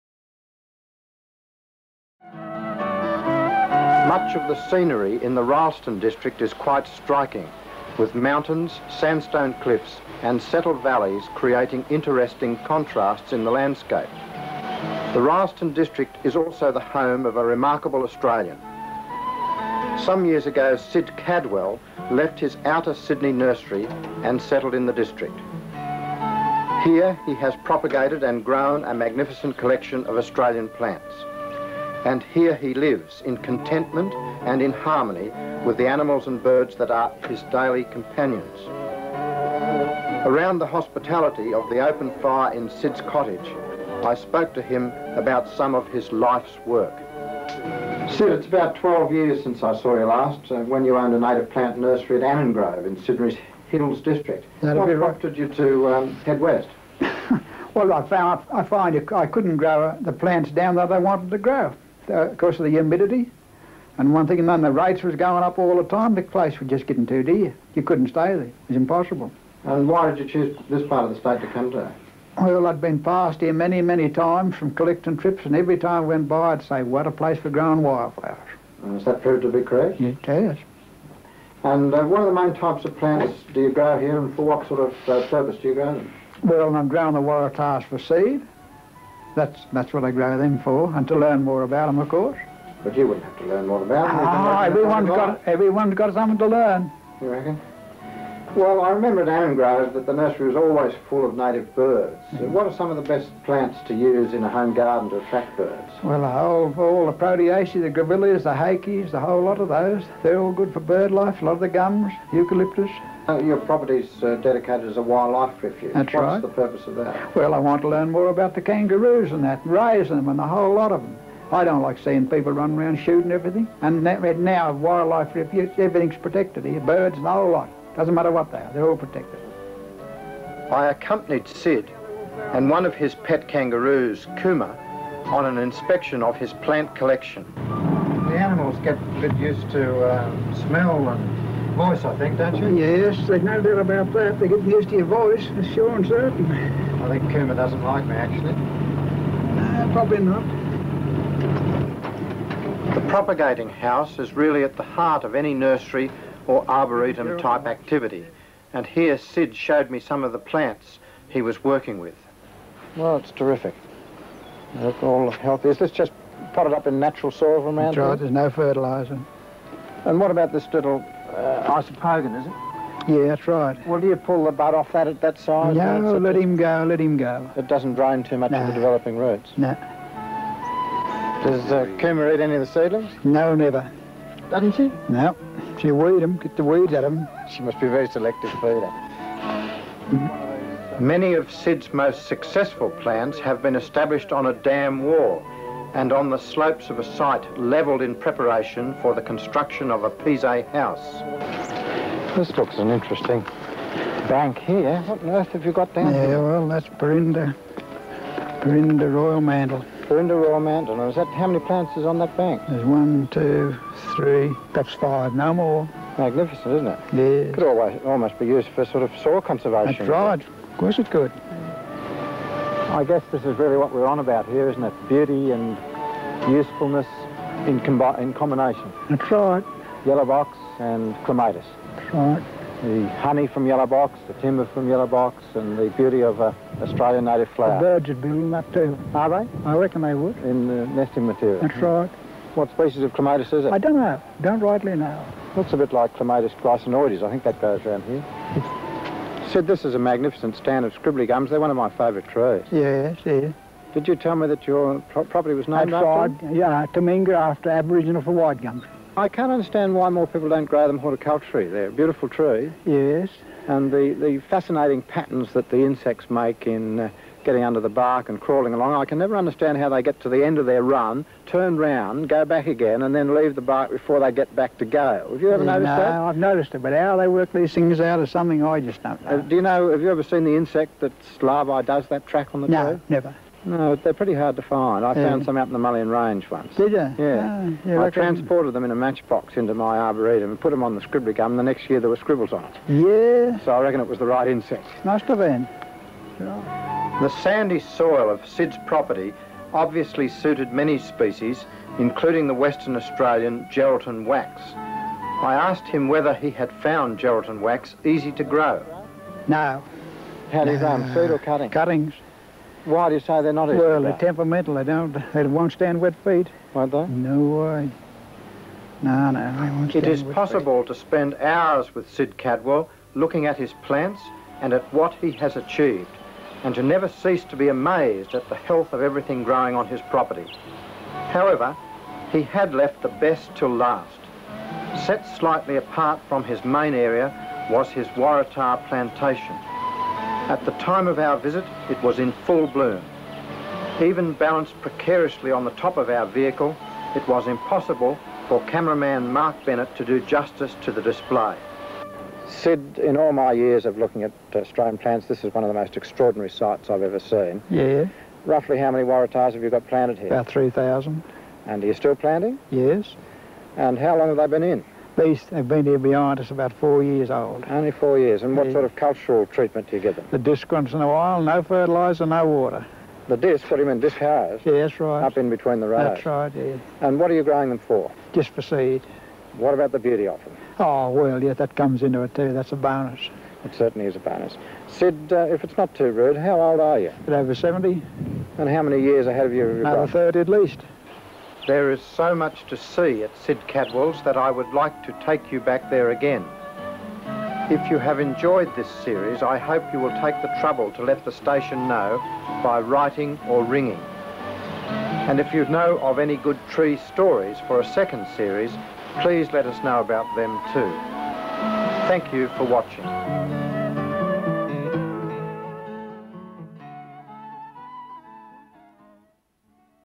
"Man of Trees". Sid Cadwell.
Sid Cadwell was interviewed in his native plant nursery near Rylstone NSW by Chris Pratten in 1984.